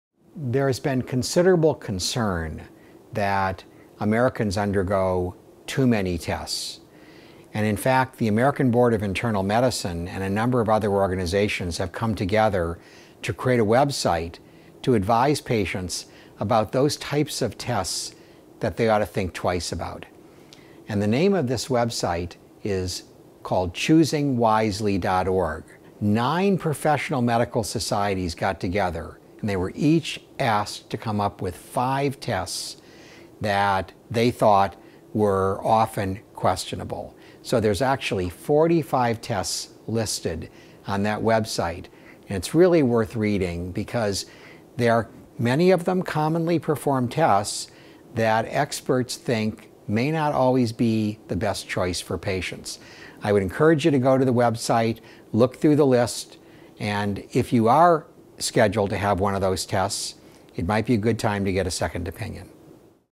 Heart Tests - Questionable Medical Tests
Which heart tests are required and which are optional? Dr. Steven Nissen, MD, discusses the state of medical testing in the US and provides resources to see what heart tests you really need.